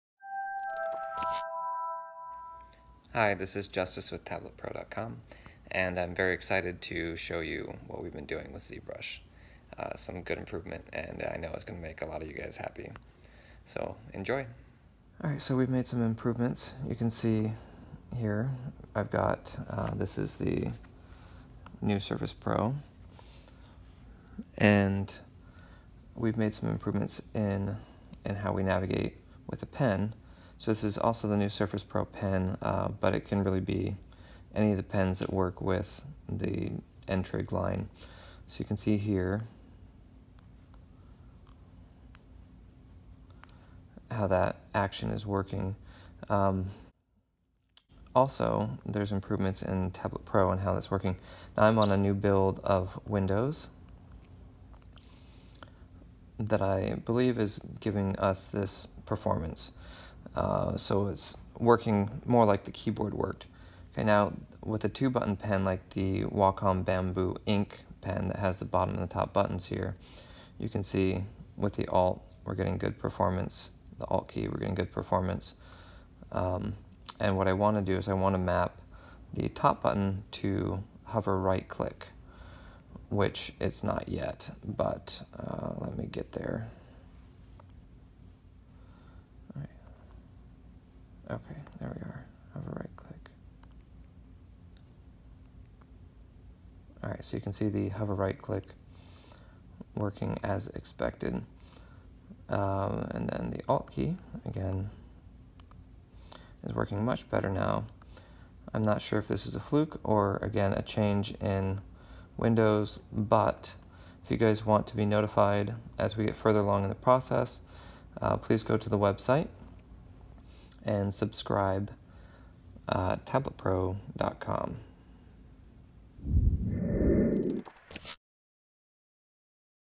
Zbrush on Surface Pro 2017 Bamboo Ink Tablet Pro perfect navigation test
Huge improvements, I think navigation is working correctly now or very close. If you want to be informed of the new improvements in zbrush on a ntrig tablet like the Surface Book, Surface Pro or Surface Studio (also Vaio tablets) please subscribe on our website. Getting close!!! Special thank you to my friend Leo Reyes for helping with the pen script. Leo you are a genius!
Our New easy guide to install Tablet Pro
Buying a computer? Consider using this link! (I get paid if you buy anything)